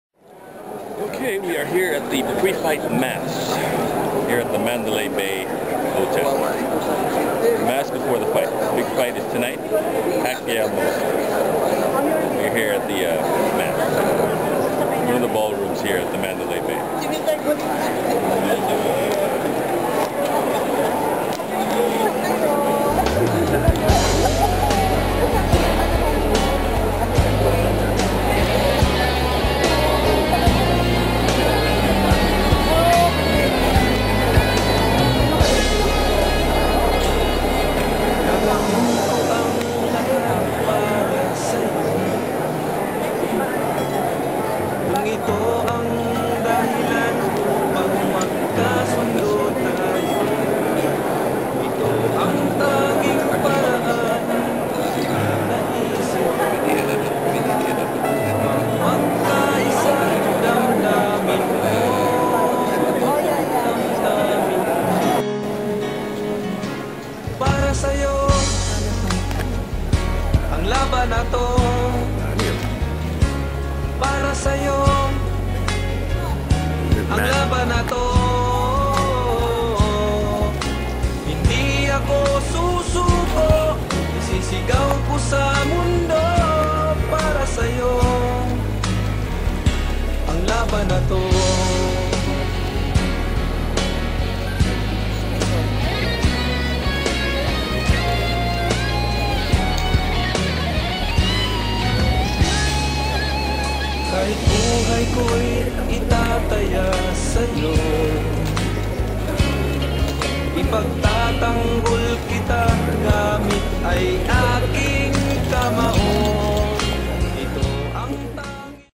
Pre-fight mass with Manny Pacquiao at Mandalay Bay Las Vegas - May 7, 2011
The morning of the big welterweight fight between champion Manny Pacquiao and contender "Sugar" Shane Mosley they held a mass at the Mandalay Bay resort hotel and I was lucky enough to attend. Unlike past years when the mass was held simply in Manny's hotel room, this time it was held at one of the big ballrooms at Mandalay Bay to accomodate al the people and well-wishers that attended.
In addition to Manny Pacquiao, many other celebrities were in attendance that morning, including former Manila mayor Lito Atienza, Ilocos Sur governor Chavit Singson, ABS-CBN sports reporter Dyan Castillejo, Vicky Belo, Hayden Kho and many more!